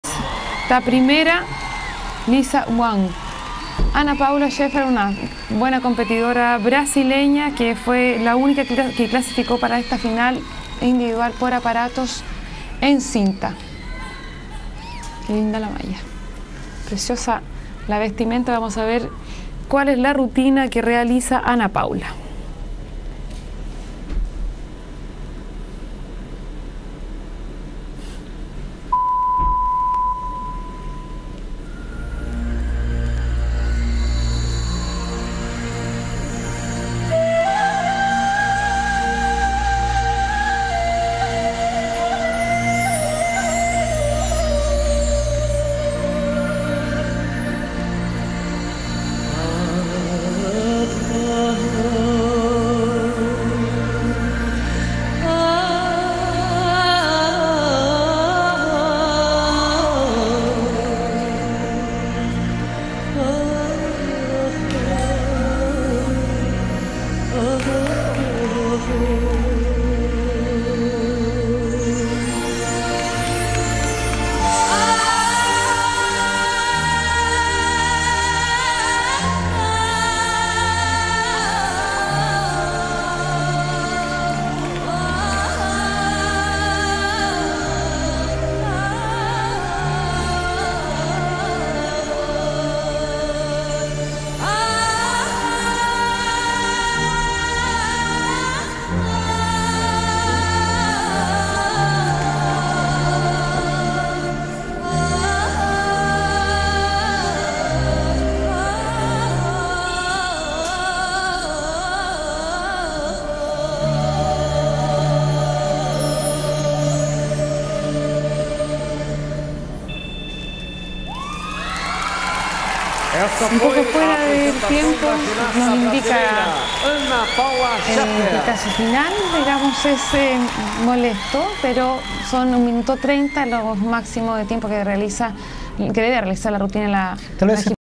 Ana Paula Scheffer BRA - Fita @ Rio 2007
Ana Paula Scheffer se apresenta com o aparelho fita no pan do Rio 2007